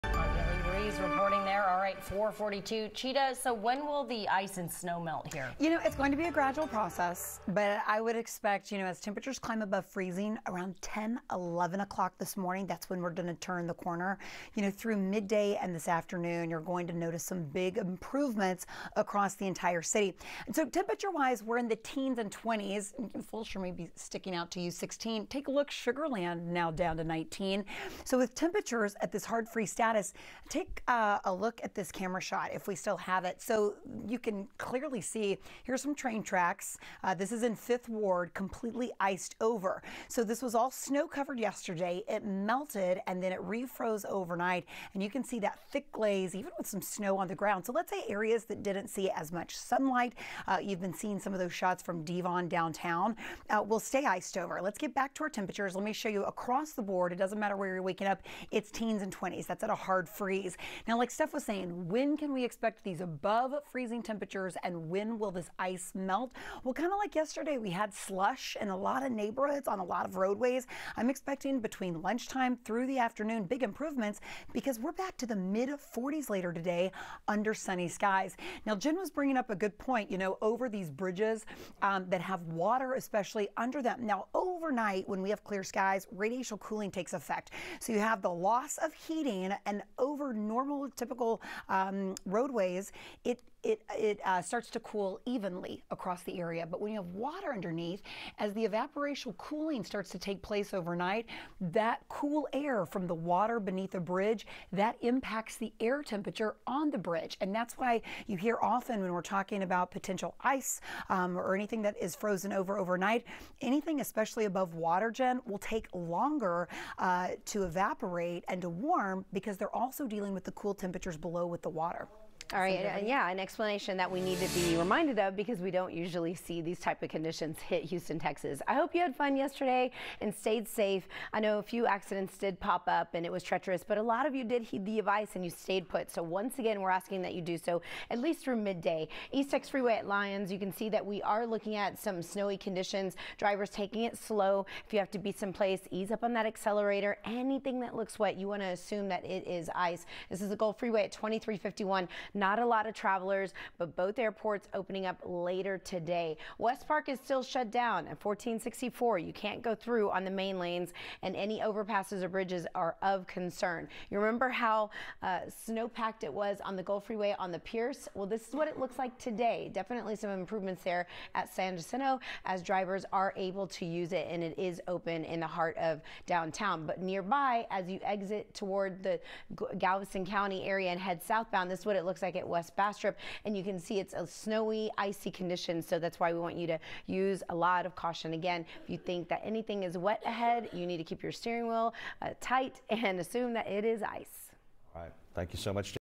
What you need to know about icy bridges and overpasses
Meteorologist Chita Craft explains how ice forms on bridges and overpasses making for hazardous road conditions.